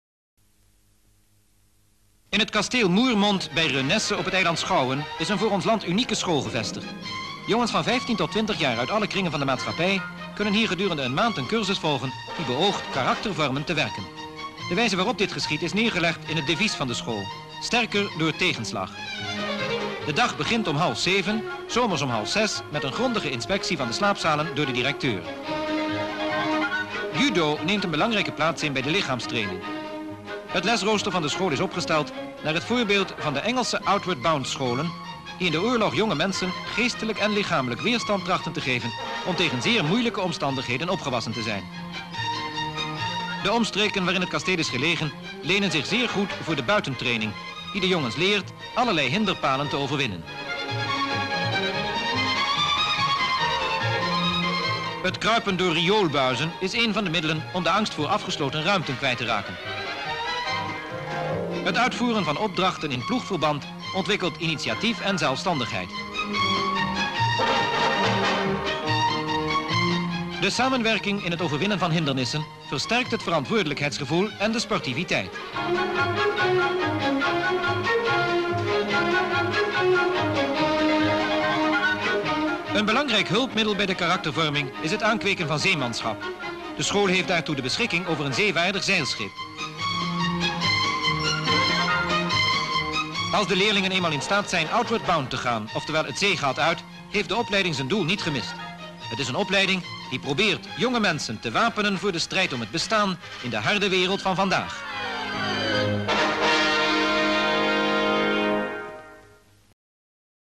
Unique school in castle Moermond near Renesse (Week number 62-02) 🔴
Description (more): Boys at the age of 15-20 take the month long course "Stronger through setbacks", which is supposed to build their character, at a school in the castle Moermond. In the morning the dormitories are inspected, after which the youth do physical exercises like judo, take an obstacle course and sailing.